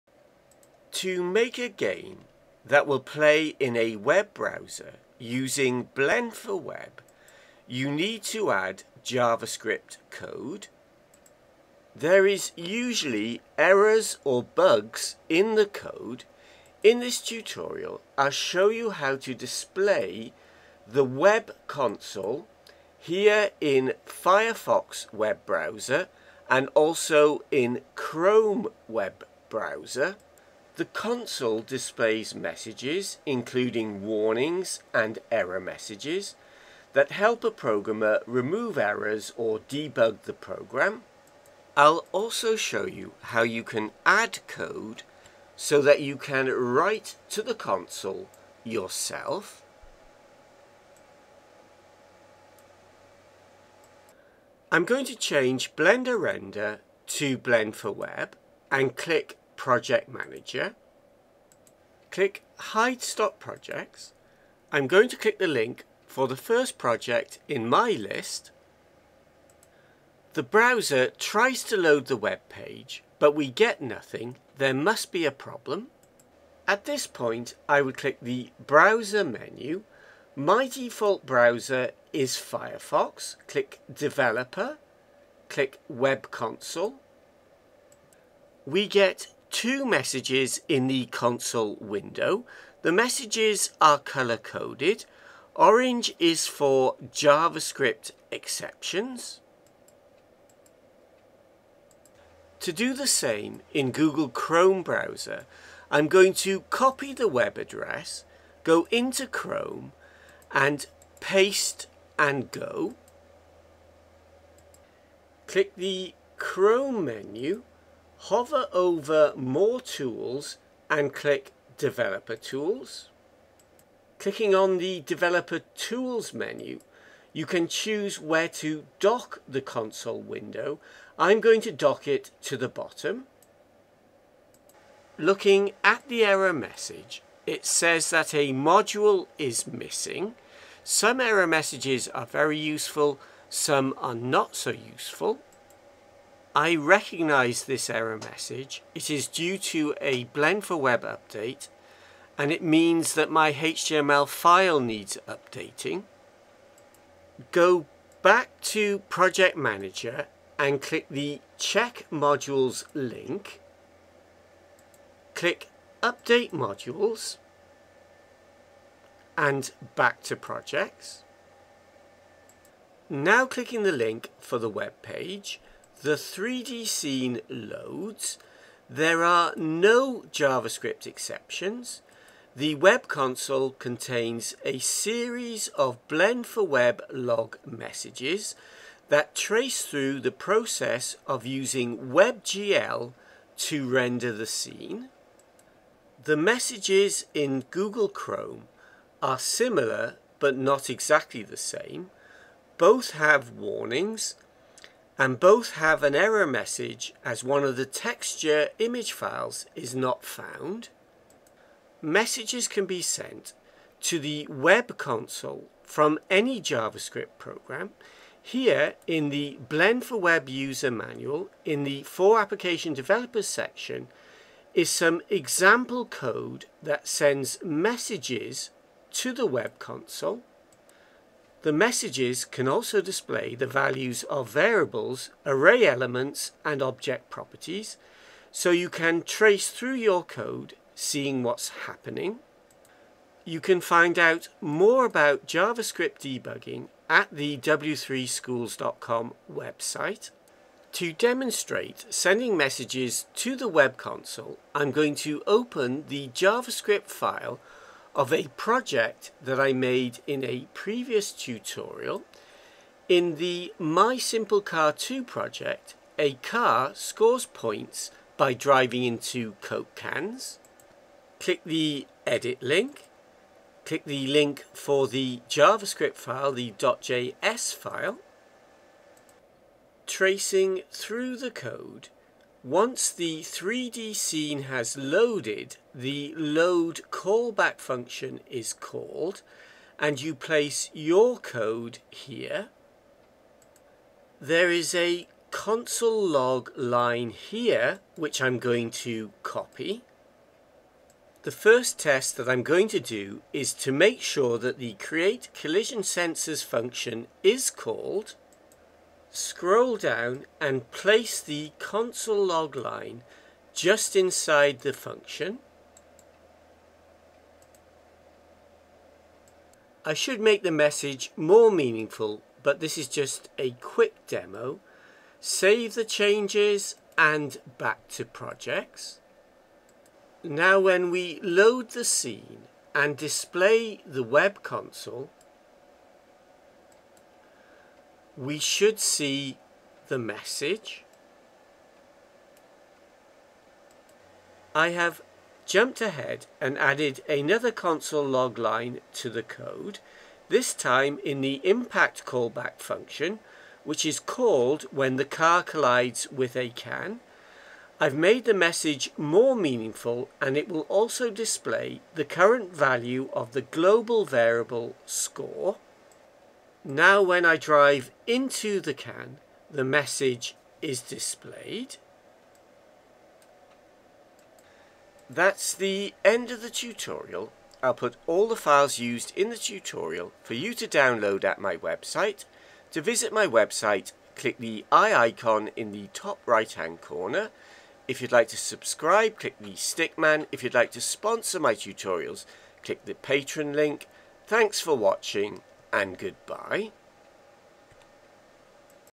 3D Apps/Games Basic Debugging Using A Browser’s Web Console, A Blender & Blend4Web Tutorial
This tutorial shows how to do basic debugging for web based 3D apps (e.g. games) made using blend4web.

This tutorial shows how to display the web console window in both Firefox and Chrome web browsers.

This tutorial shows how to write messages to the console from your app's code. The messages can display the current values of variables and properties, allowing a programmer to see what is going on.

The JavaScript command is -

console.log(“total = “, total);

Find out more about debugging at -

The software used is Blender and blend4web.  Blender is a free open source 3D software package. Blend4web is an extension to 3D Blender that provides tools and an API for putting Blender content onto the Internet.